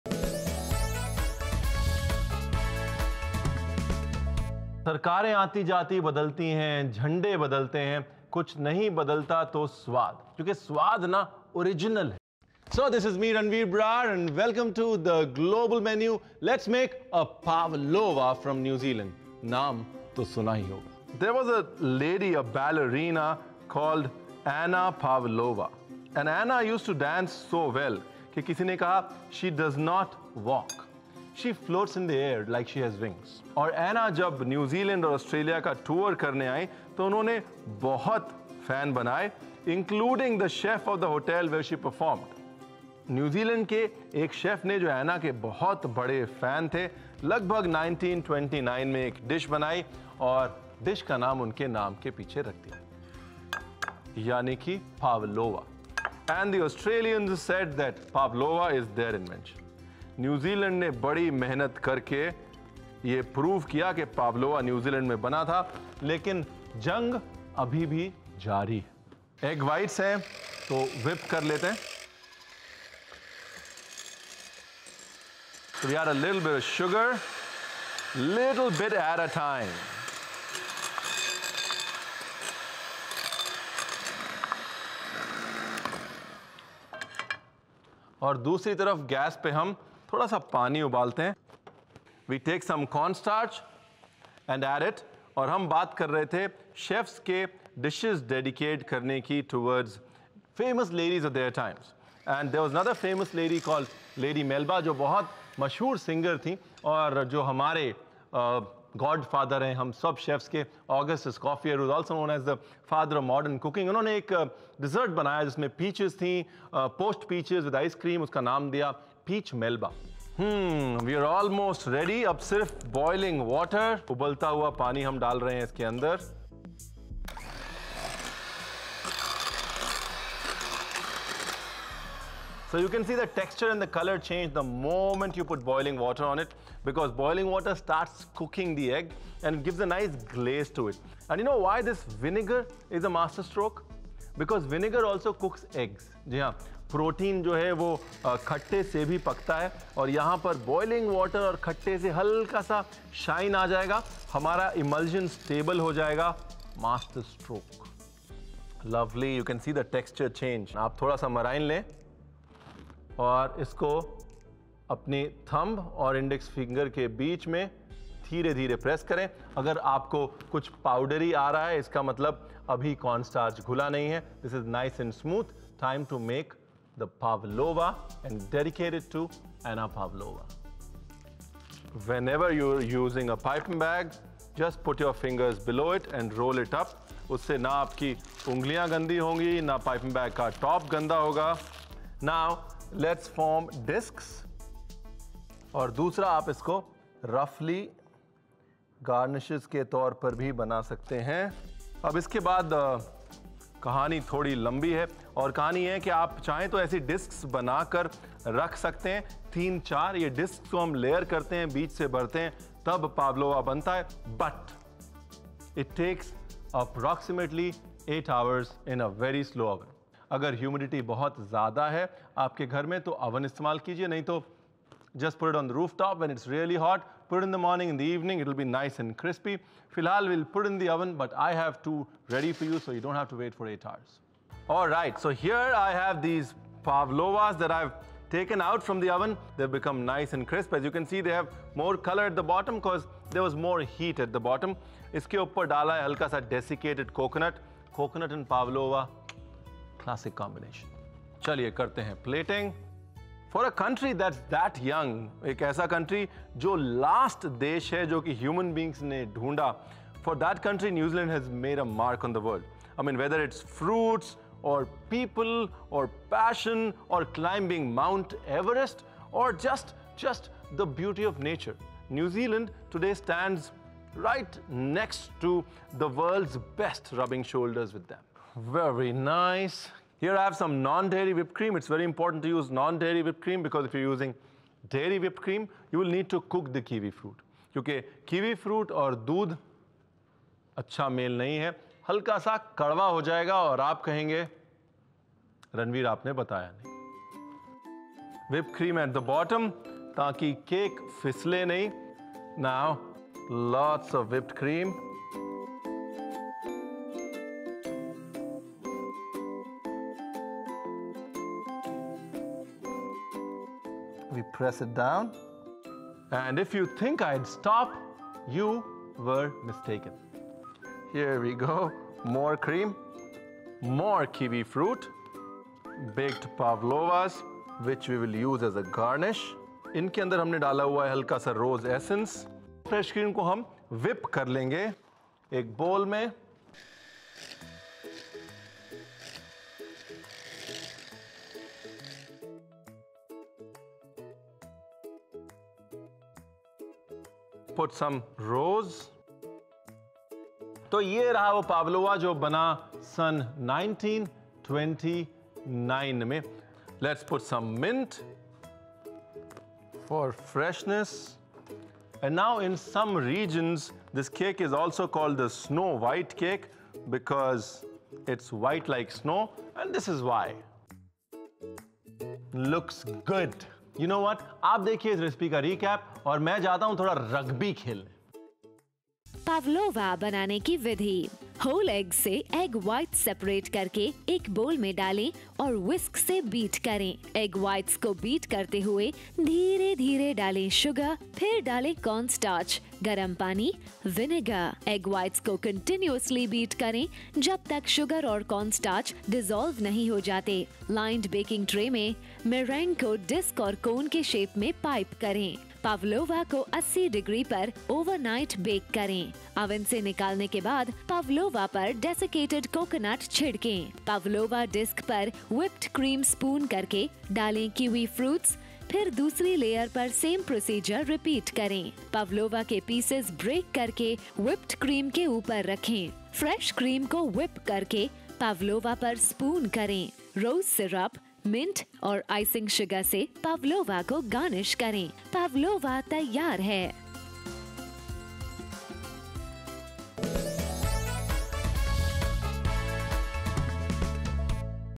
Pavlova Recipe with Chef Ranveer Brar
Pavlova is a meringue-based dessert named after the Russian ballerina Anna Pavlova. It has a crisp crust and soft, light inside, usually topped with fruit and whipped cream. The name is pronounced /pævˈloʊvə/, or like the name of the dancer, which was /ˈpɑːvləvə/.
The dessert is believed to have been created in honour of the dancer either during or after one of her tours to Australia and New Zealand in the 1920s. The nationality of its creator has been a source of argument between the two nations for many years.
'The pav' is a popular dish and an important part of the national cuisine of both Australia and New Zealand, and with its simple recipe, is frequently served during celebratory and holiday meals. It is a dessert most identified with the summer time, and popularly eaten during that period, including at Christmas time; however, it is also eaten all year round in many Australian and New Zealand homes.
Break 4 eggs carefully, separate
egg whites in a clean bowl and whisk
the egg whites, add 220 gms sugar
and add 10 gms cornstarch
Add boiling water into the bowl and
beat the eggs further, add 5 ml vinegar
and beat the eggs till cornstarch
dissolves completely
Line a baking tray and pipe meringue
in circular motion to form a large disc
With the remaining meringue
pipe small conical shapes
Bake Pavlova at 80°C for 8 hours
till it dries out, add desiccated
coconut to Pavlova once out of the ove
On a cake tray place Pavlova and
spoon whipped cream, drop kiwi fruits
Place another disc on top of whipped
cream and repeat the process
Add large broken pieces of baked
Pavlova on top of the cream
Whip fresh cream and spoon it on
Pavlova pieces, drizzle rose syrup,
add mint sprigs and sprinkle icing sugar
Pavlova is ready to be served.